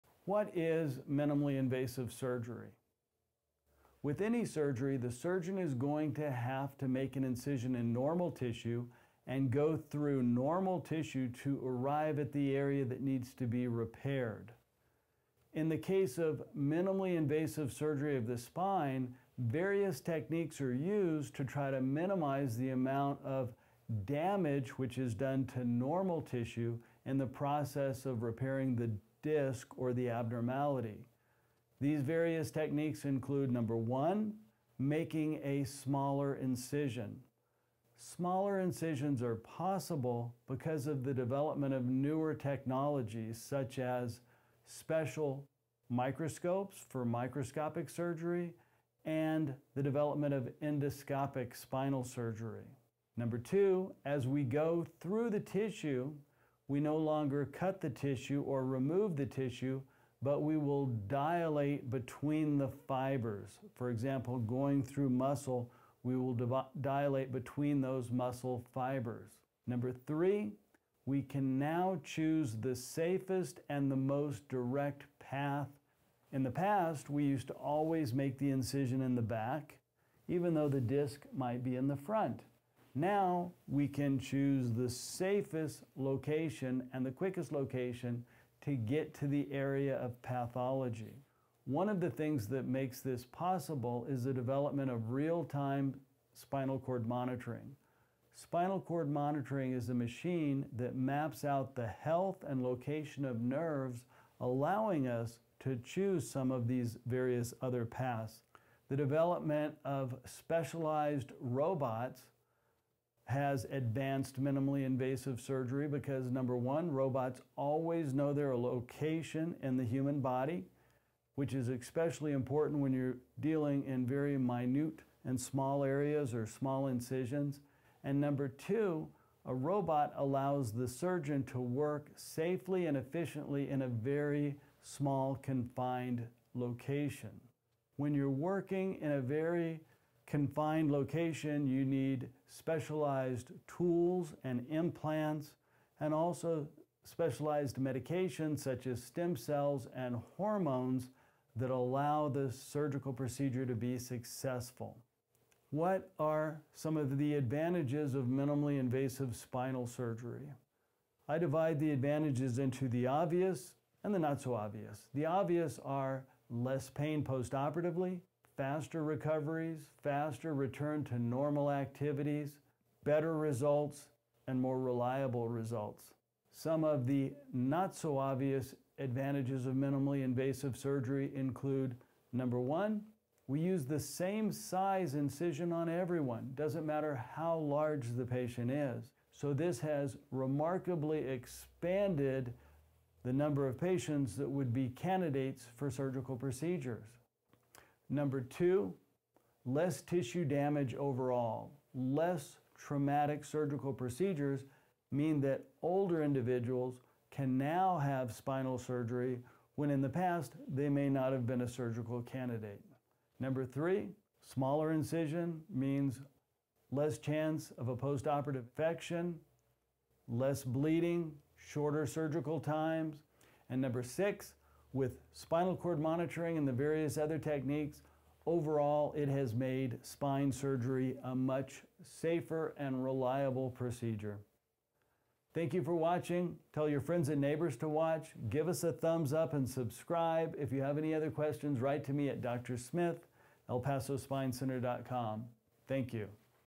What Is Minimally Invasive Lumbar Surgery?
What is minimally invasive lumbar surgery? A short video with animation by Dr. Dean E Smith MD who has performed tens of thousands of successful minimally invasive spinal surgeries.